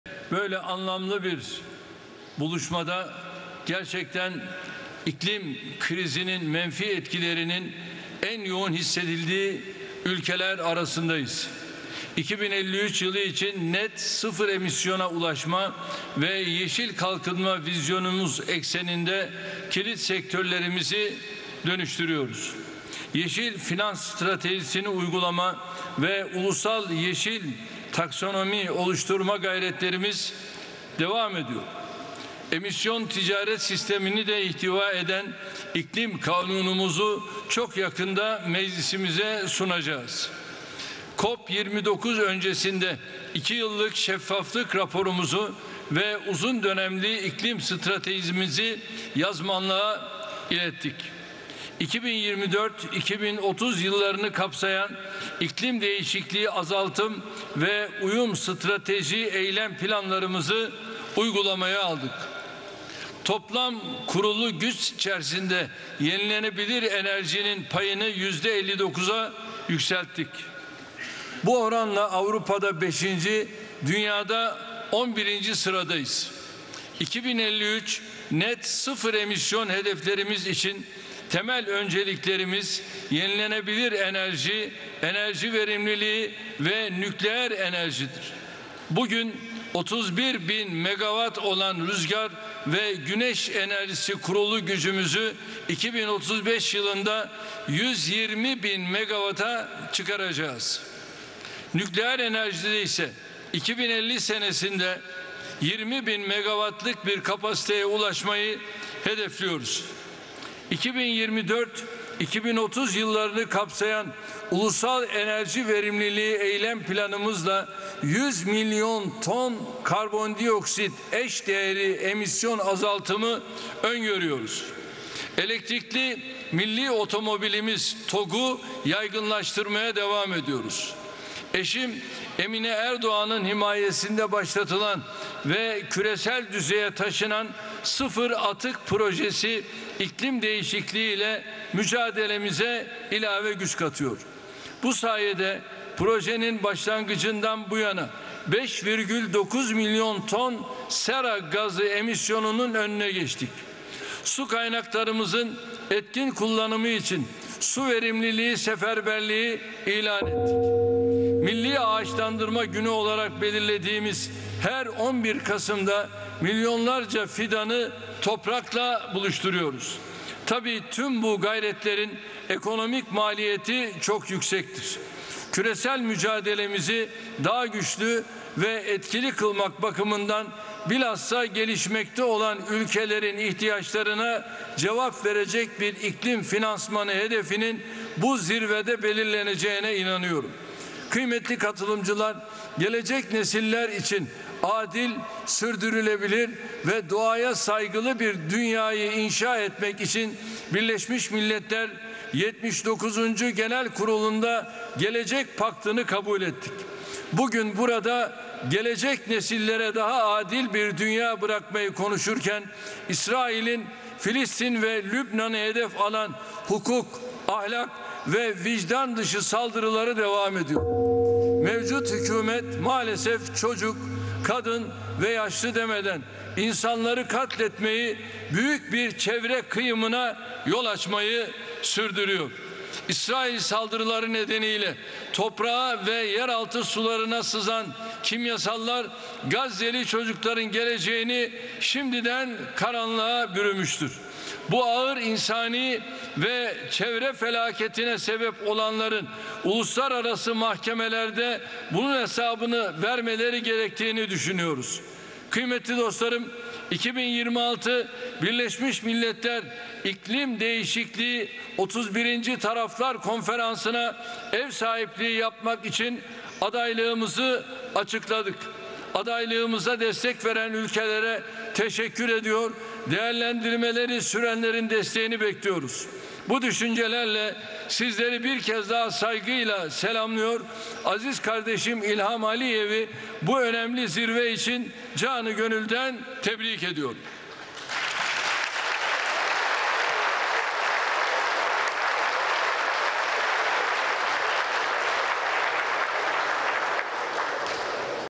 Erdoğan Azerbaycan'da Konuştu: Kardeşim Aliyev'i Tebrik Ediyorum
Cumhurbaşkanı Erdoğan, Azerbaycan'da düzenlenen Dünya Liderleri İklim Zirvesi'nde konuştu; "Aziz kardeşim İlham Aliyev’i bu önemli zirve için canı gönülden tebrik ediyorum"

Türksat 4A
Frekans : 12.103
Sembol: 8333
Polarizasyon: Yatay ( Horz )
Modulasyon: 8SPK